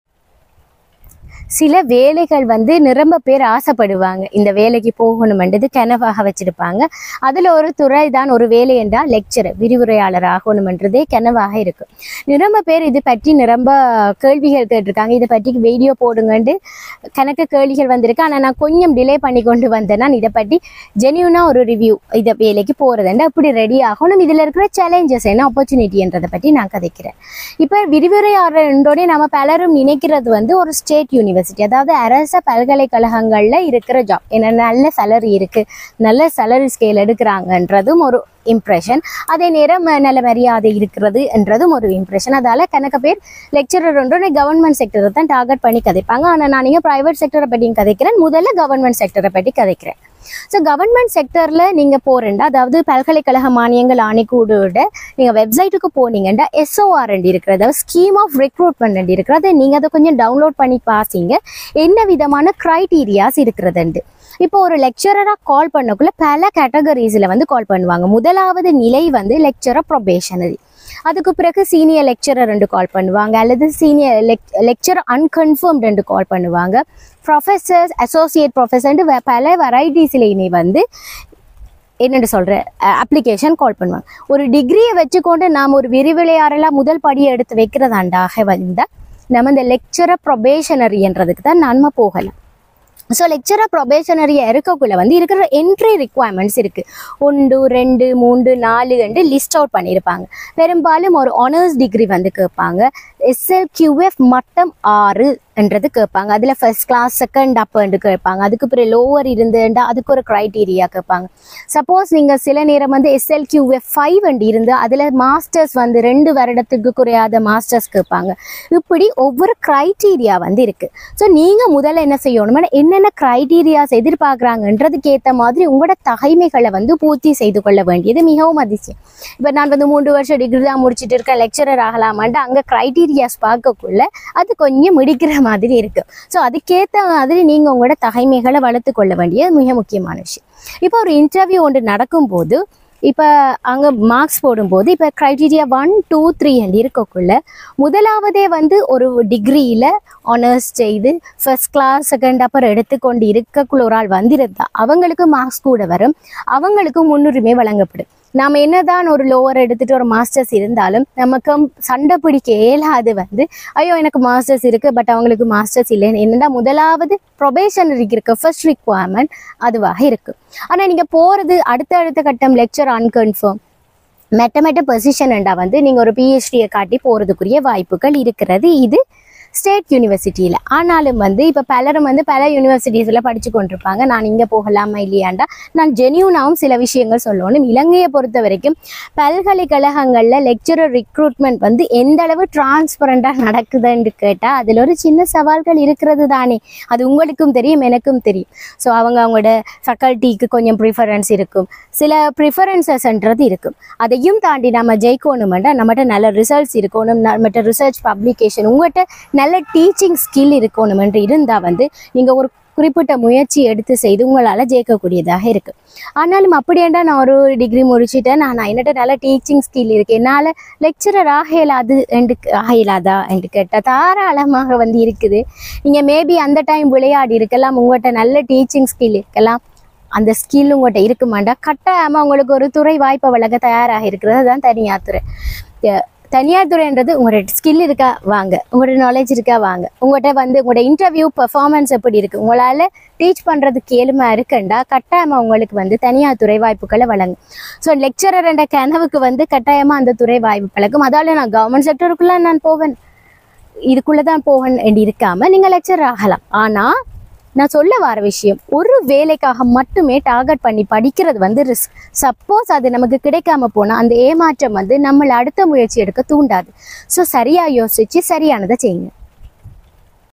How to become a lecturer in Sri Lanka??விரிவுரையாளராவது எப்படி?
This video is presented by Mrs. Saranya Lavapirathan, she has more than six years of experience in the field of higher education.
She worked as  Assistant Director at The Open University of Sri Lanka's Ampara study center. She has experience working as a demonstrator at both the University College, Rathmalana and the Sri Lanka Advanced Technological Institute (Hardy, SLIATE).
She has four years of experience working in the corporate sector. She earned a merit pass in her MBA program at London Metropolitan University, a postgraduate degree in science and technology of herbal products from the University of Sri Jayewardenepura, and a first-class B.Sc. in agriculture science from the Eastern University of Sri Lanka.
In addition she is also a certified life coach with a qualification from the Sri Lanka Institute of Training and Development.
She is currently employed as  manager at Esoft Metro Campus Batticaloa